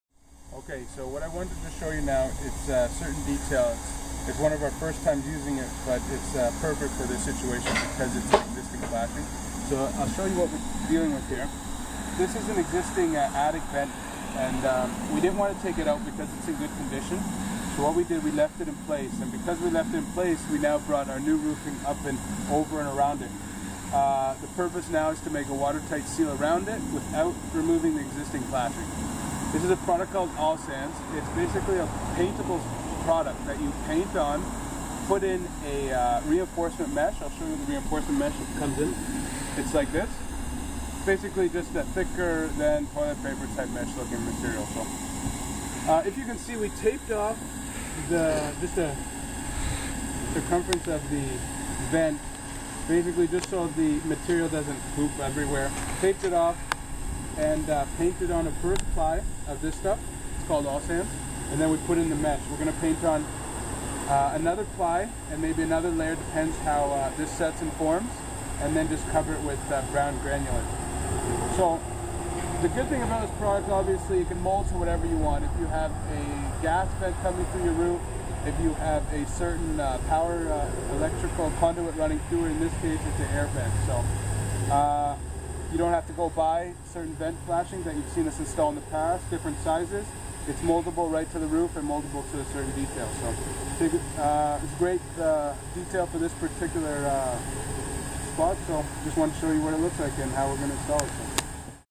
How to Create Water Tight Seal Without Flashing: Legacy Flat Roofing & Sheet Metal
Legacy Flat Roofing & Sheet Metal How To Video:  Create a Water Tight Seal without the use of metal flashing

Sometimes on a job there are air vents, pipes, conduit etc that require metal flashing to give extra water leaking protection.  More often than not because of the age of the roof these items need to be replaced, or they need metal flashing.  But in this case here as you can see, the air vents in this home are in excellent shape and don't require metal flashing.

So we use a special compound called All Sands, which is an easy to apply, paintable product that we use with a reinforcement mesh backing.  It's a perfect compound that molds to whatever you need and is as water tight as metal flashing.